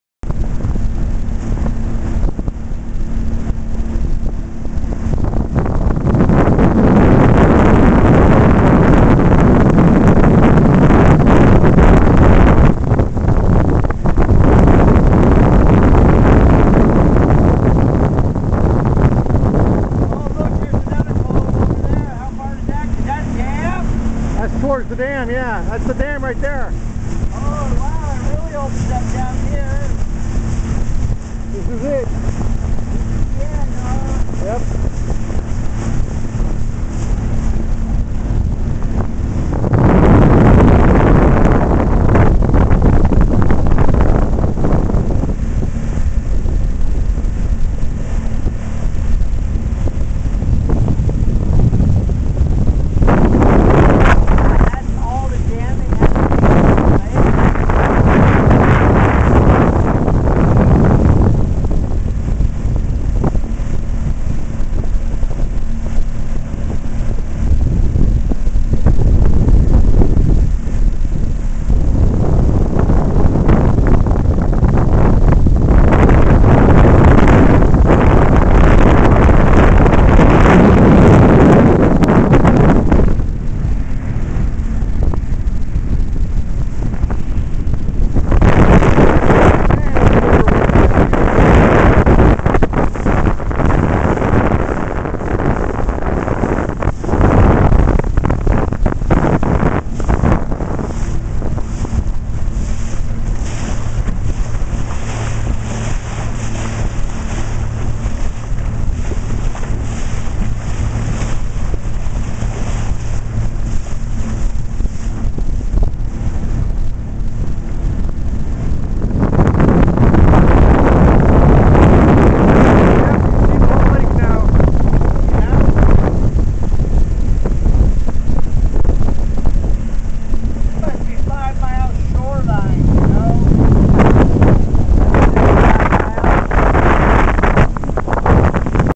Gregg and Rod out on Del Valle Lake 6/9/2009 2222
Gregg and Rod out on Del Valle Lake 6/9/2009 on pontoon boat